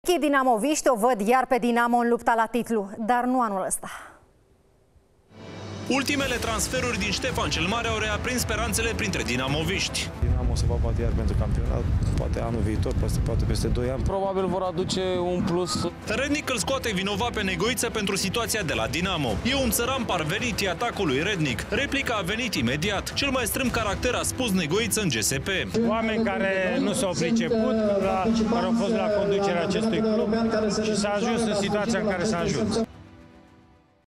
Atacuri uluitoare între Mircea Rednic şi Ionuţ Negoiţă | Știri din sport
"E un ţăran parvenit!" vs. "Cel mai strâmb caracter!" Atacuri uluitoare între Mircea Rednic şi Ionuţ Negoiţă